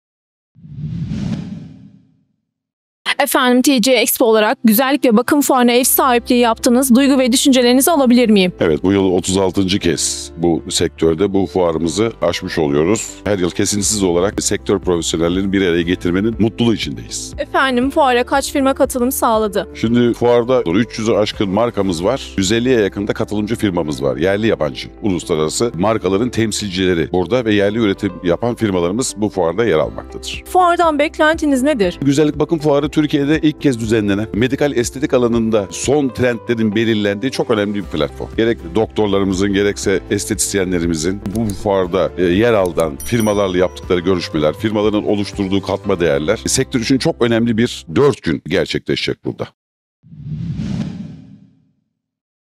TG Expo Proje Koordinatörü Gökhan Büyükataman
TG Expo Proje Koordinatörü Gökhan Büyükataman, Güzellik ve Bakım Fuarı'nda Fuar Dergisi mikrofonlarımıza özel açıklamalarda bulundu.